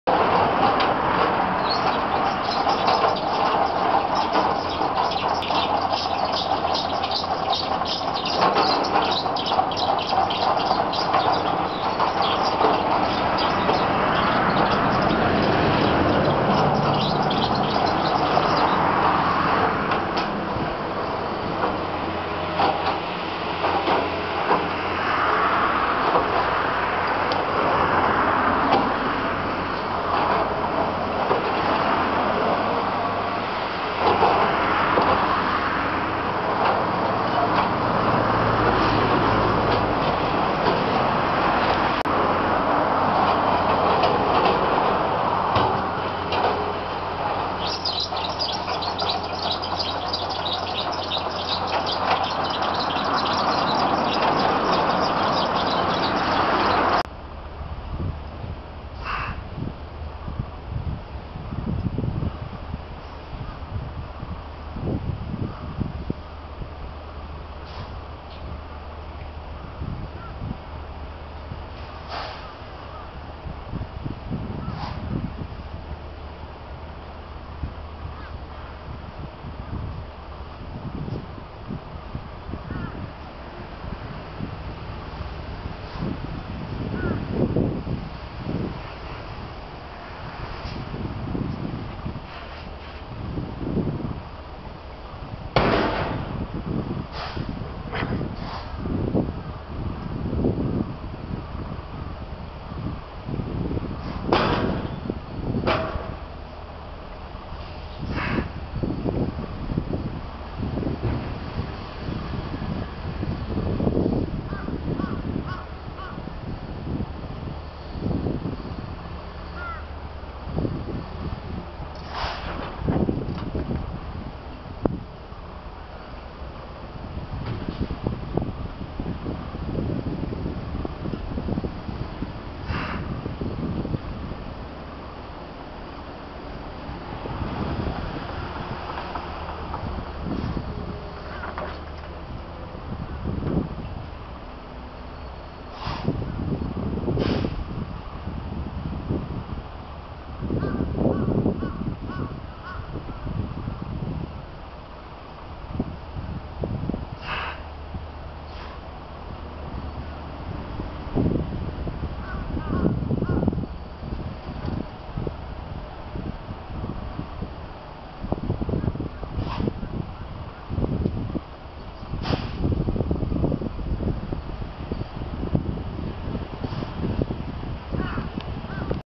新井田川護岸工事H24 8 10金
八戸市内の新井田（にいだ）川にかかる柳橋上から上流側。ダンプカー通るたびに上下に揺れてあせった。
1.　42秒　～11:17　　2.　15秒　～11:18　　3.　2分2秒　　～12:01　　奥に見えるのは住金鉱業の石灰石サイロとそれにつながるトンネル。